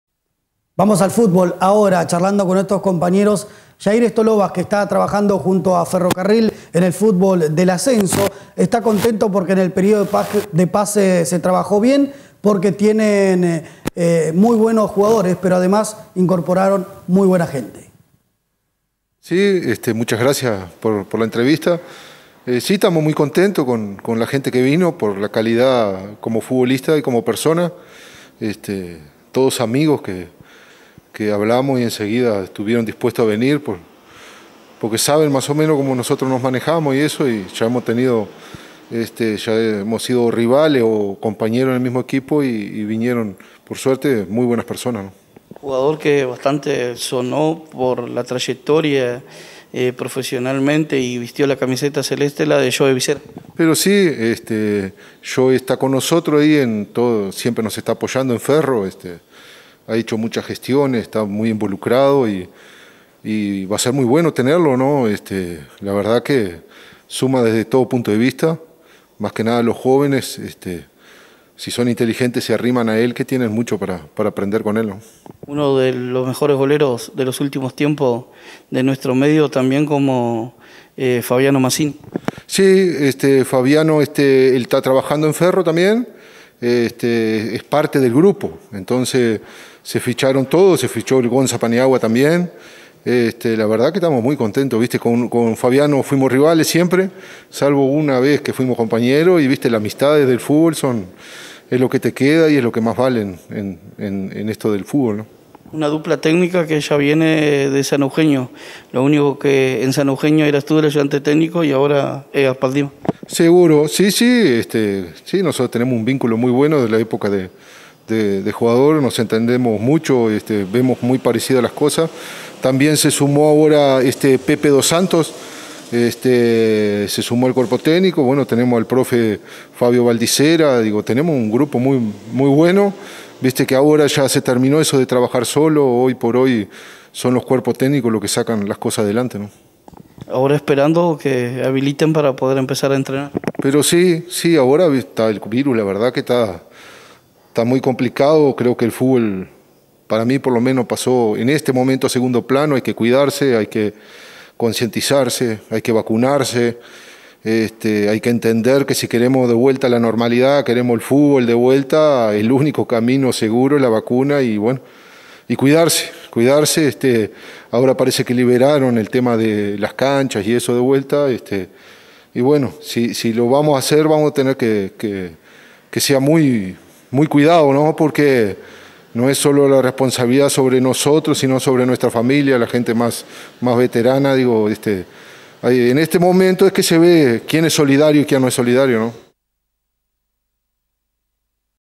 Infotres - 12/03/2021 - Yair Stolovas
Ferrocarril se prepara para volver a jugar en la Divisional B